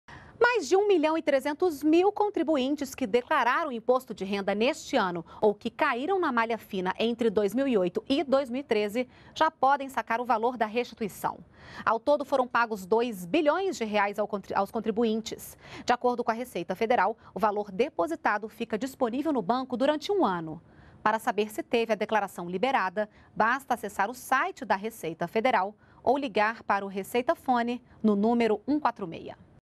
Mais de 1,3 milhão de contribuintes já podem sacar a restituição de 2014
NBR NOTÍCIAS - 16.06.14: Mais de 1,3 milhão de contribuintes que declararam o Imposto de renda 2014 ou que caíram na malha fina, entre 2008 e 2013, já podem sacar o valor da restituição. Ao todo, foram pagos R$ 2 bilhões aos contribuintes. Segundo a Receita Federal, o valor depositado fica disponível no banco durante um ano. ou ligar para o Receita Fone, no número 146.